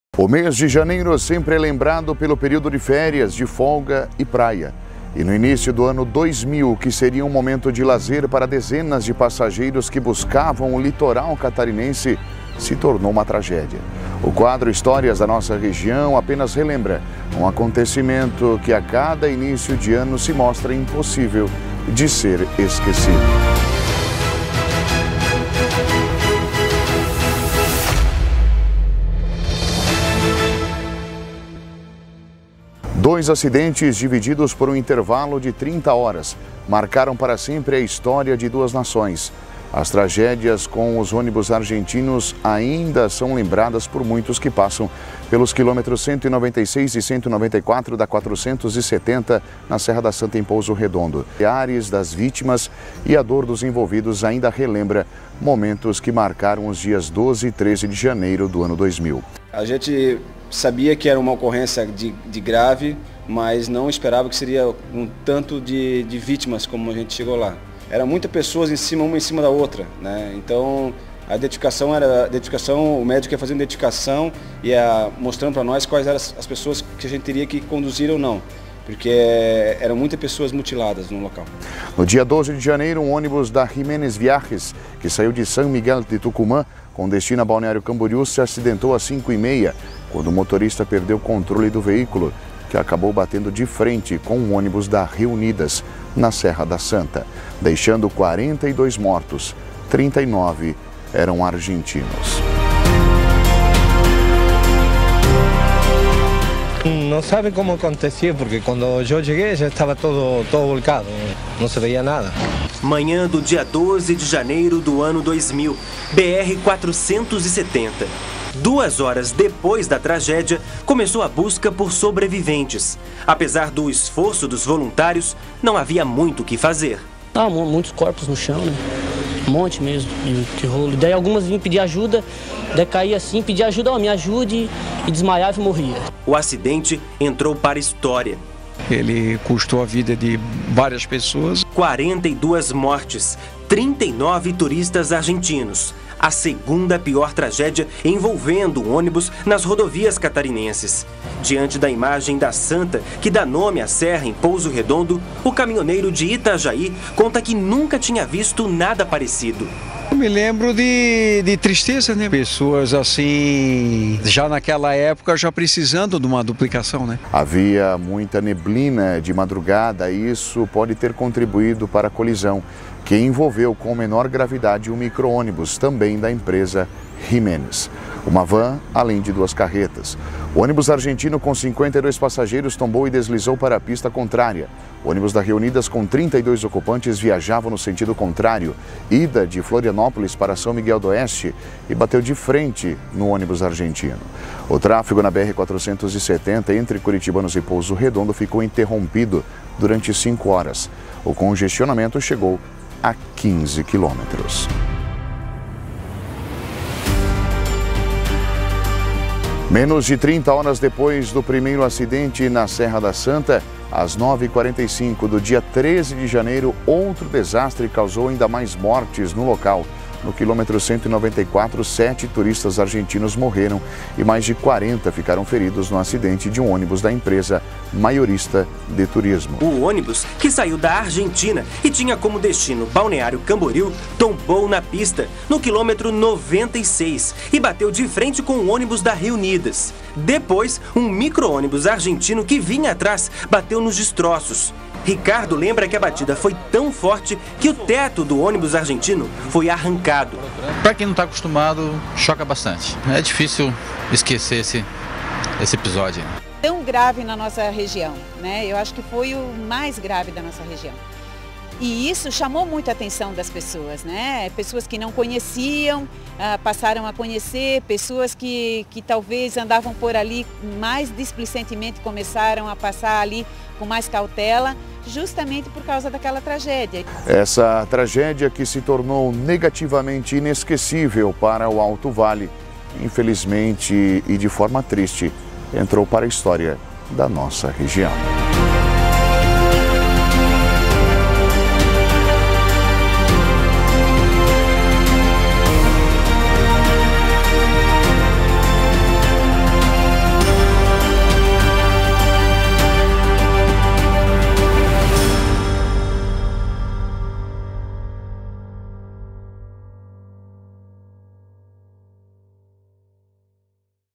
Há 24 anos, Pouso Redondo era palco de uma tragédia histórica entre Brasil e Argentina
O mês de janeiro sempre é lembrado pelo período de férias, de folga e praia. E no início do ano 2000, o que seria um momento de lazer para dezenas de passageiros que buscavam o litoral catarinense, se tornou tragédia. O quadro Histórias da Nossa Região apenas relembra, um acontecimento que a cada início de ano, se mostra impossível de ser esquecido!